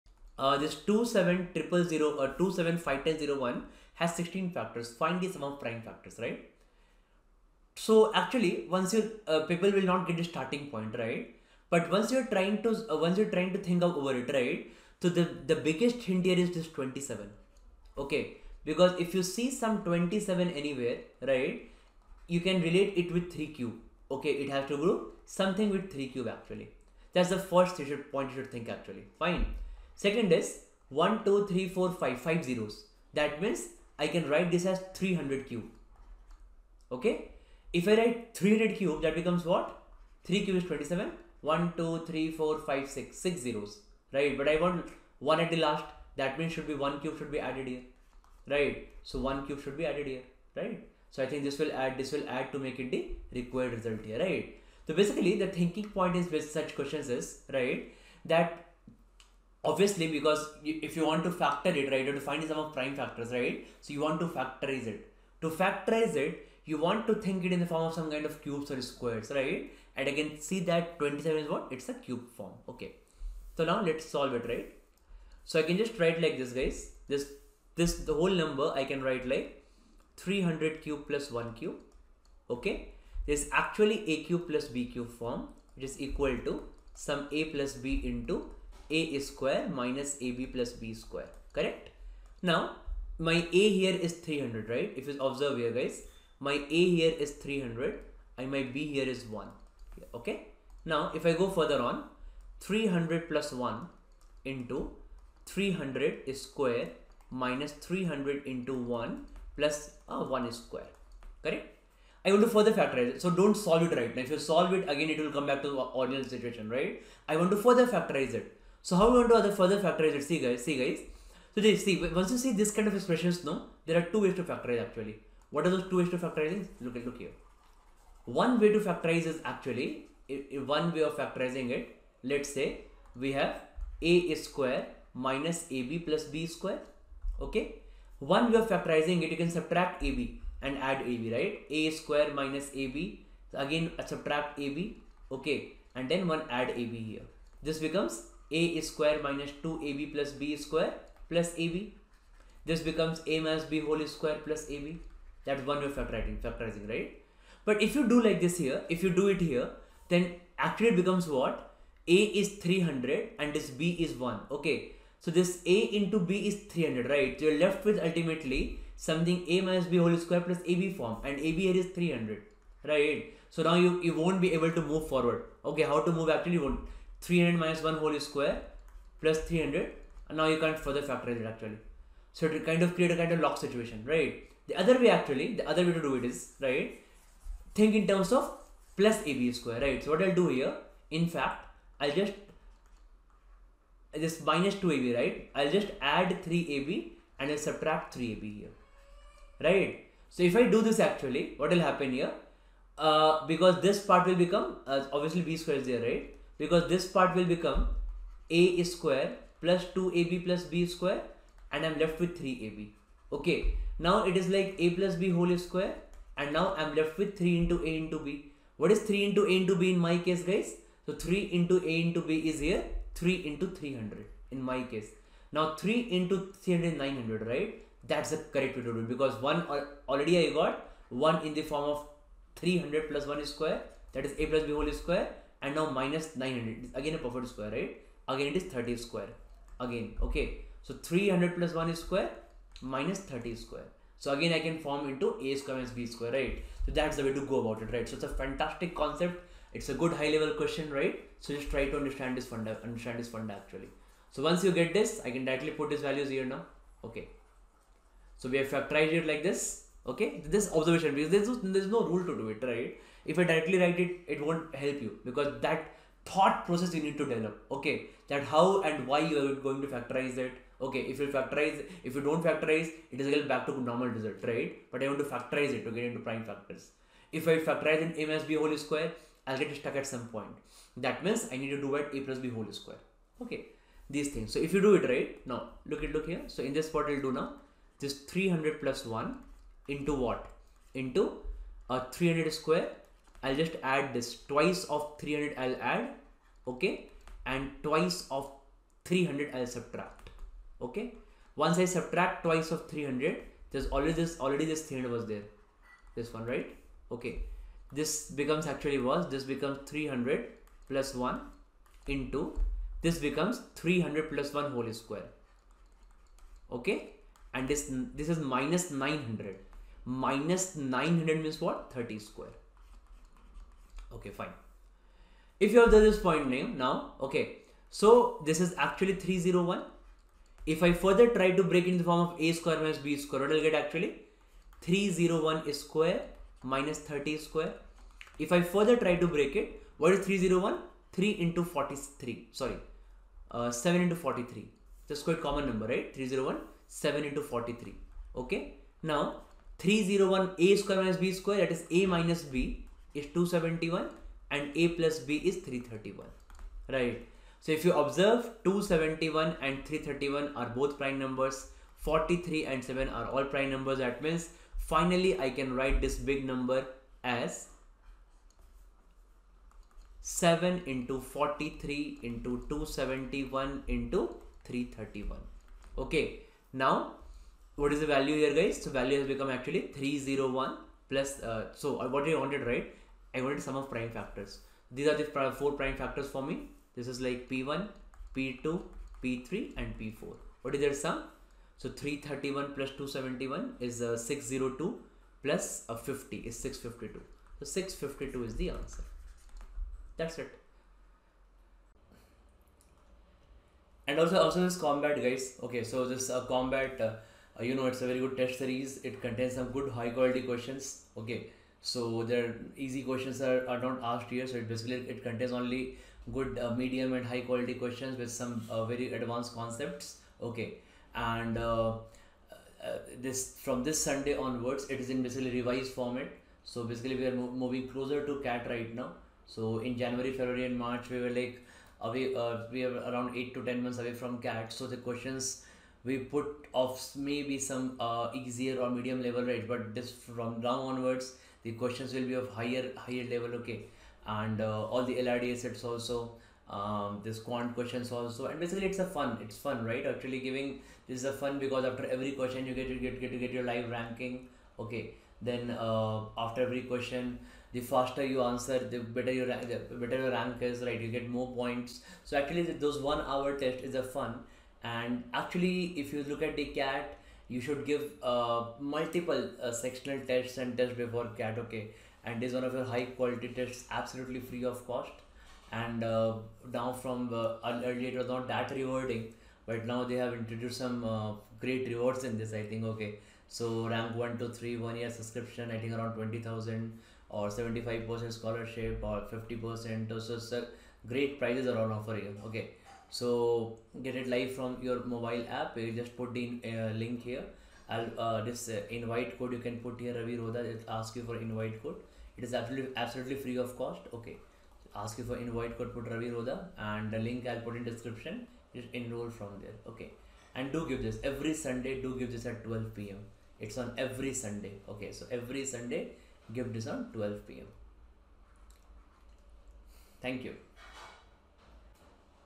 Quant Questions Practice of Advance Level - 23 II CAT PREPARATION
Still unsure of divisor-sum tricks? Ravi Prakash Sir teaches direct formulas and advanced variations of sum of factors problems for CAT 2025 and CAT 2026 Quant prep.

Enroll in Rodha's Full CAT 2025 Course (with Dedicated Doubt Support & Structured Plans)